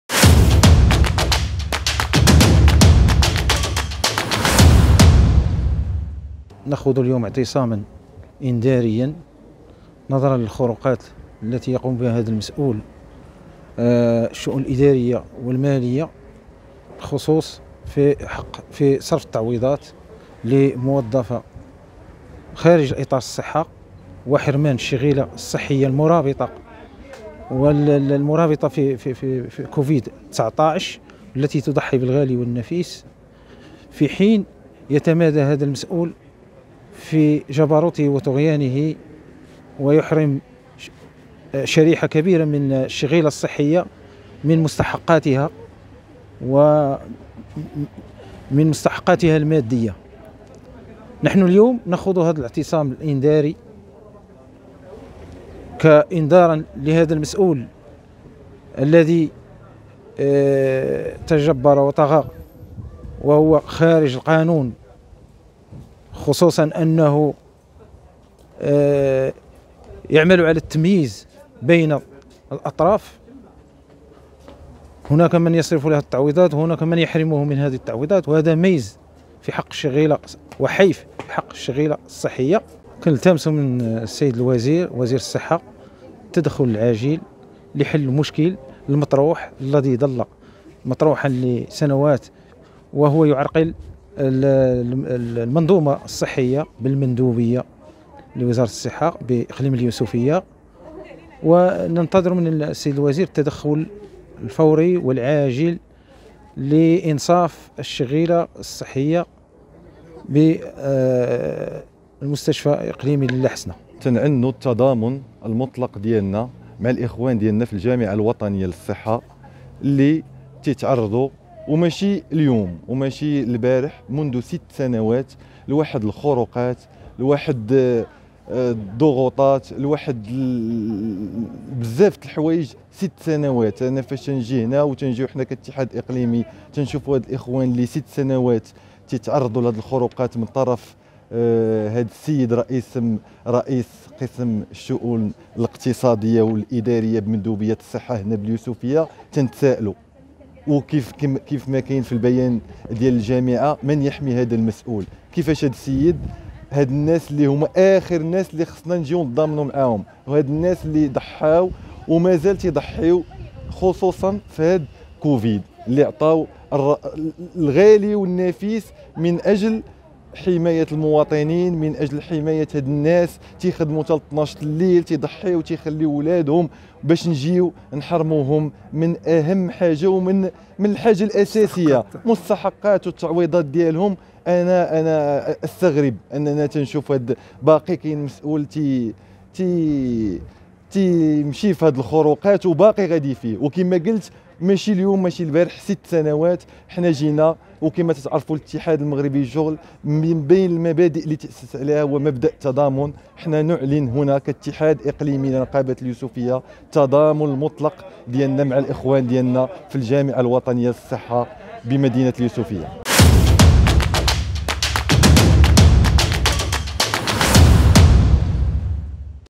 ملائكة الرحمة تنتفض ضد هذا المسؤول باليوسفية
طالبت شريحة الموظفات والموظفين بالمستشفى الإقليمي الأميرة لالة حسناء باليوسفية برفع الظلم والحيف الذي يلحقها منذ ستة سنوات من طرف رئيس الشؤون الإدارية والإقتصادية حيث توج ذلك بعدم صرف تعويضات ملائكة الرحمة ومستحقاتهم السنوية في الوقت الذي استفاد بعض التقنيين (ات) من تعويضات استنكرتها بعض النقابات

أخبار المغرب
# أخبار عاجلة